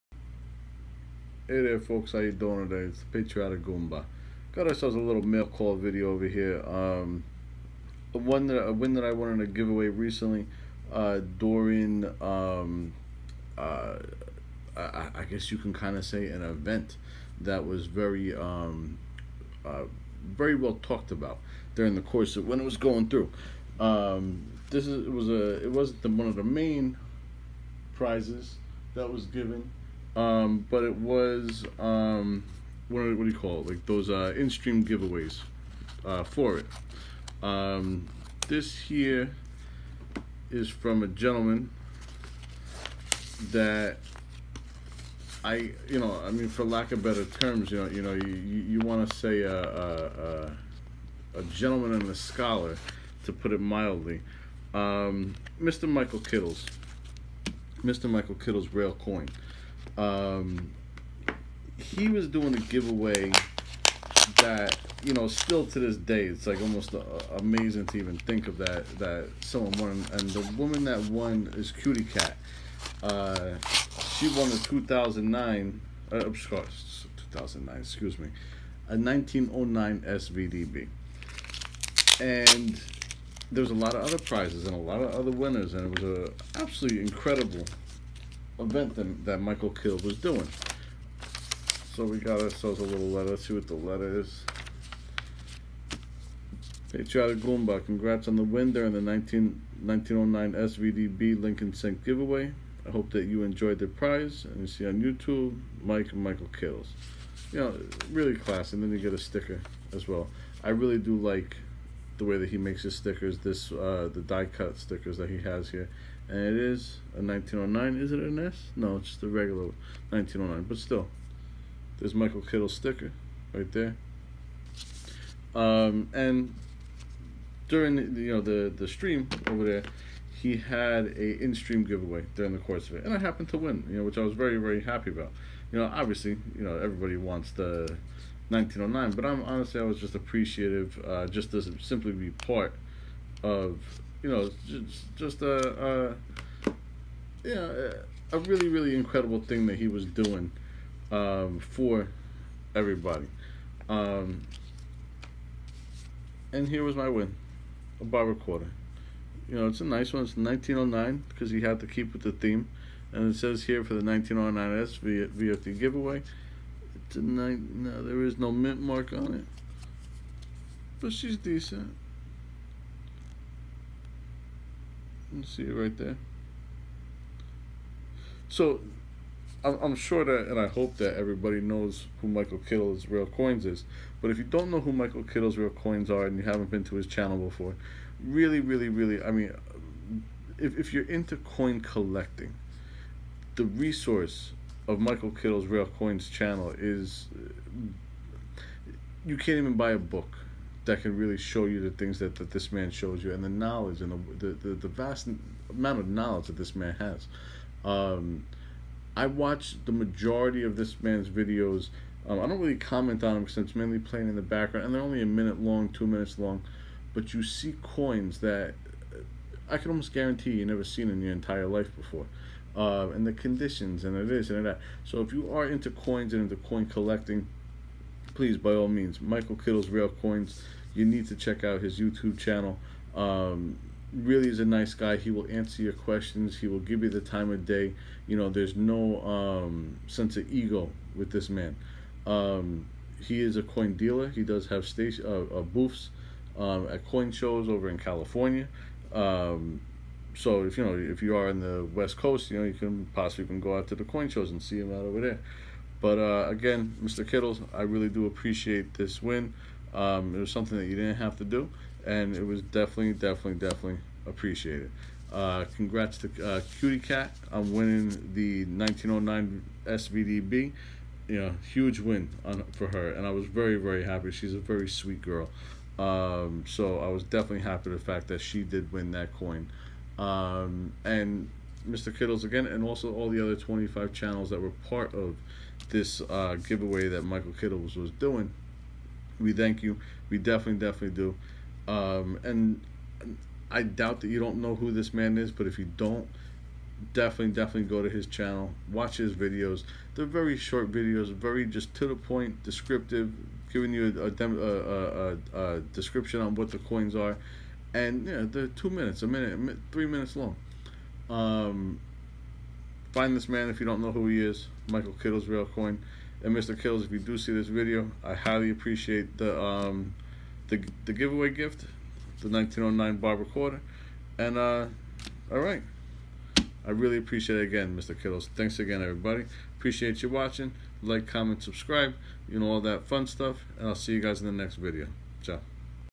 Michael Kittle Rare Coin’s: Mail Call GAW
Thanks You Mr. Kittle on this fantastic 1909 Barber Quarter. It’s Truly Appreciated